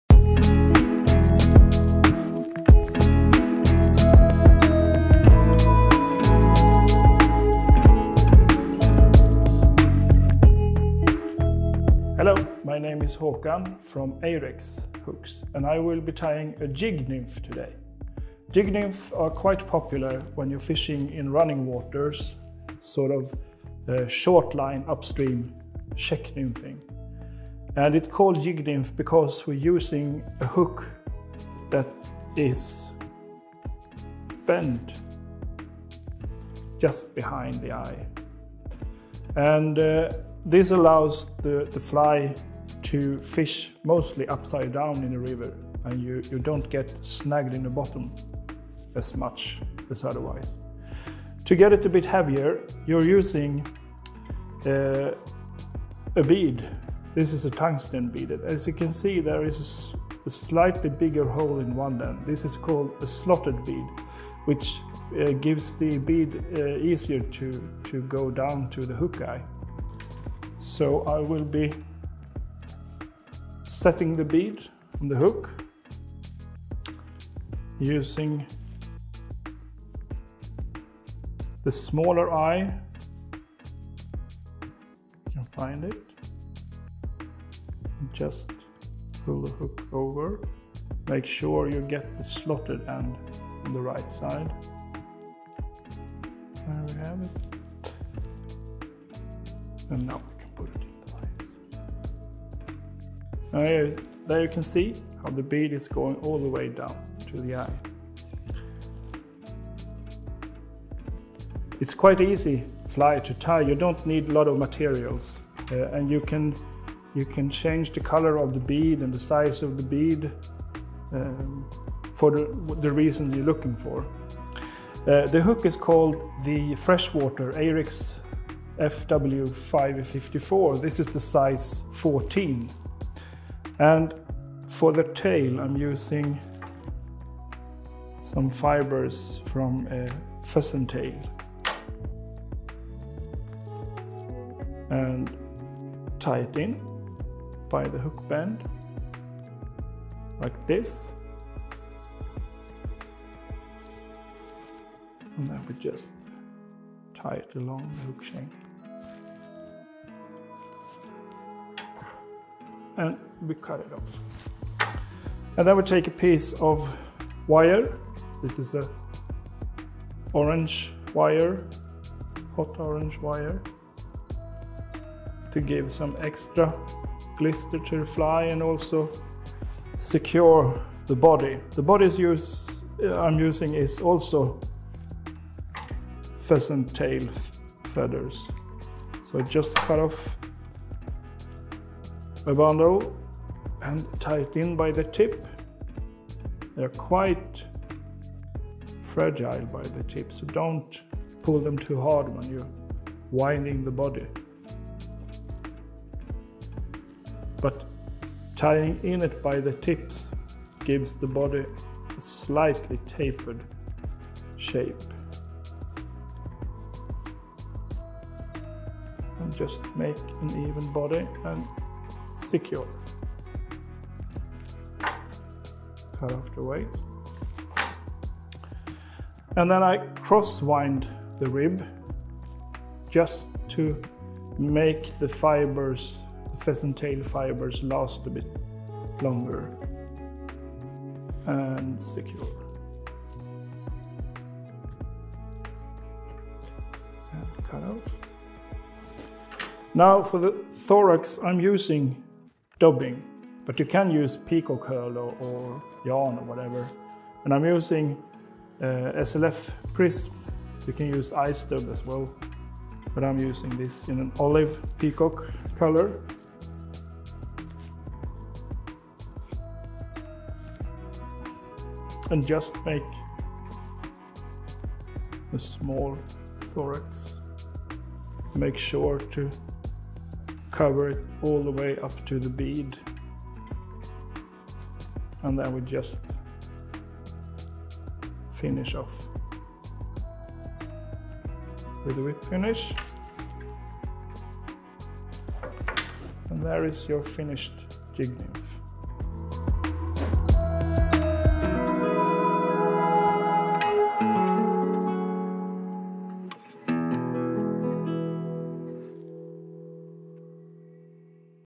Ahrex - Red Head Pheasant - by Håkan Karsnäser
Heavy nymphs is just plain effective - no doubt about that. But the traditional nymphs often get caught between snags and rocks in the river and streams. In this case a up-side-down nymph is the key to success, and the jig-style nymphs or czech-nymphs are invented to solve this problem. The czech nymphing style is spreading around the world, and has gained a massive popularity beeing super-effective for trout and graylings. Our very own Håkan Karsnäser will in this video show, how he easily can make a czeck-style mini jig.

Hook: Ahrex FW554 – CZ Mini Jig
Bead: Fl. orange 2,5 mm
Thread: Brown 8/0
Tail: Pheasant tail fibres
Rib: Hot orange wire
Body: Pheasant tail fibres
Thorax: Dark-olive Ice Dub